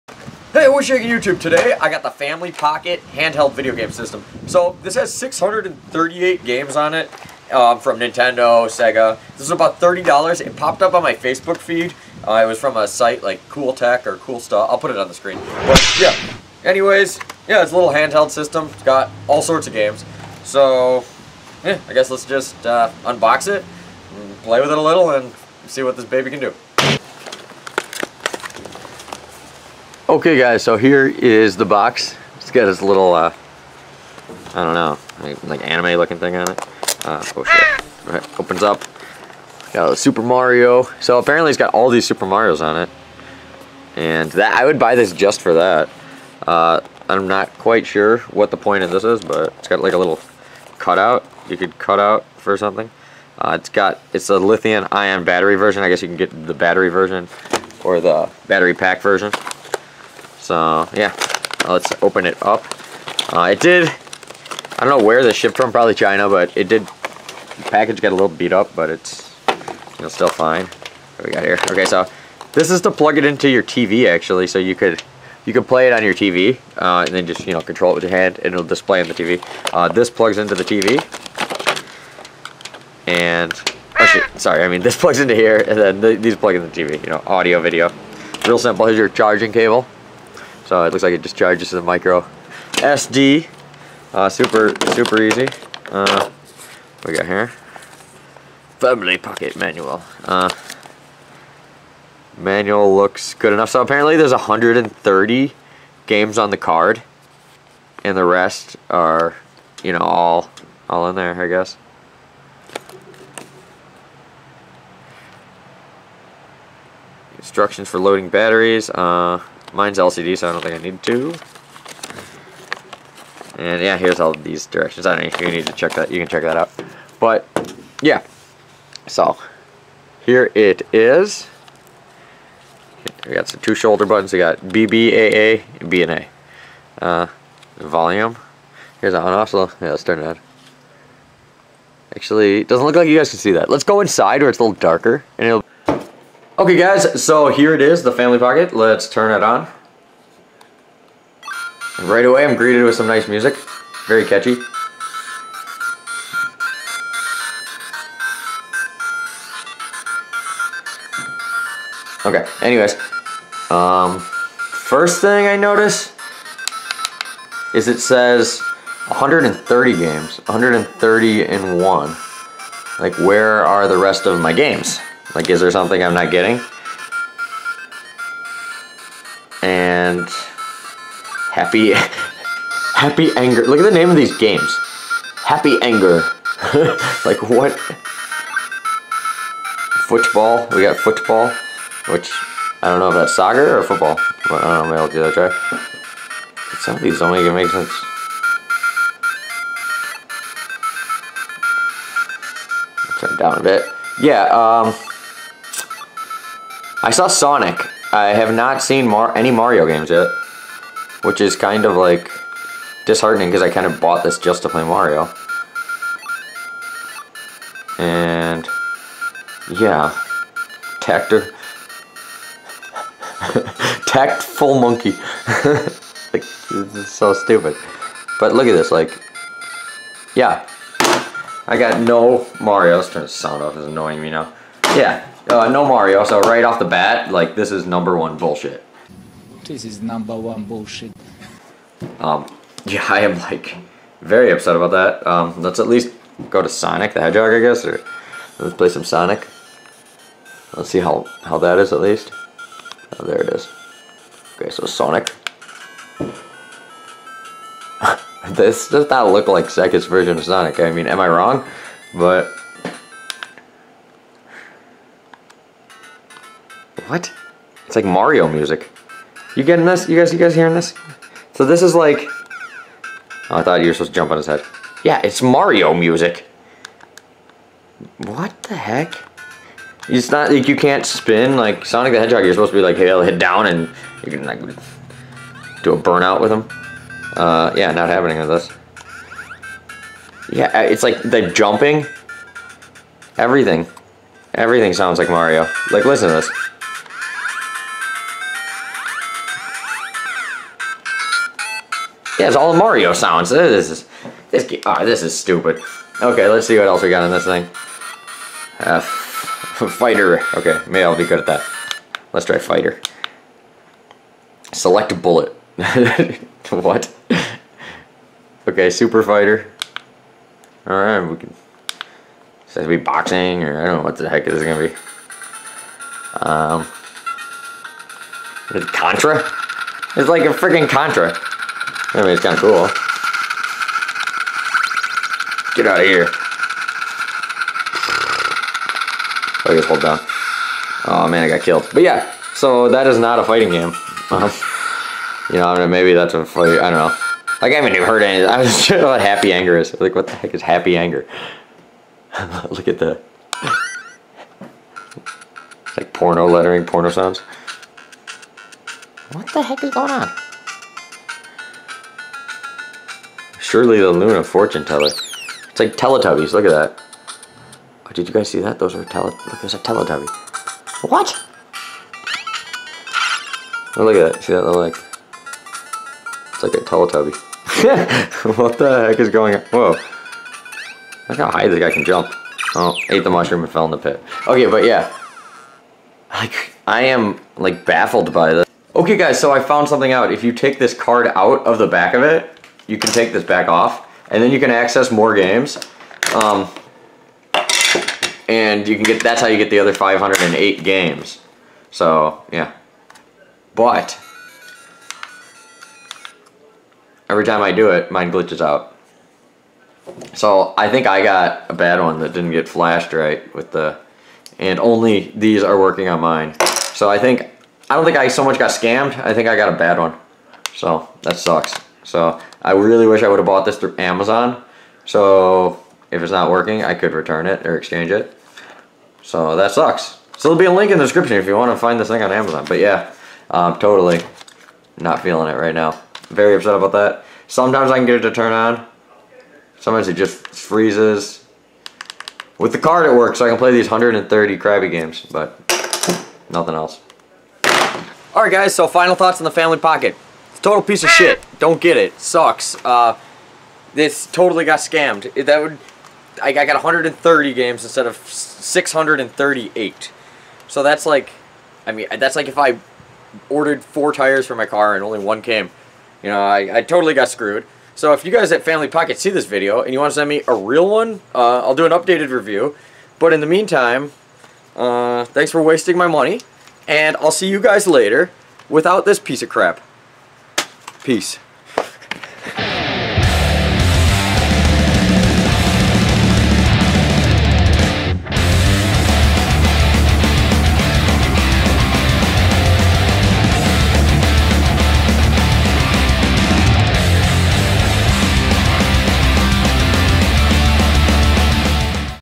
638 Game Handheld Retro Classic Gaming Console Unboxing and Review. Family Pocket Review.
Whats shaking YouTube so today I got something pretty cool this is a handheld gaming console that has 638 games in it.  So the family pocket comes with 508 games pre installed on it and a card that has 130 games on it.  As of right now I have a second one they sent me for exchange because this one had an error with the internal storage.  I will be doing an updated review with thee new one that has all the games.

Official Team Member of Magic YoYo
Tweet at me on twitter @kowalskiDylan
psn- hotdiggidy

MY GEAR
ACTION CAMERA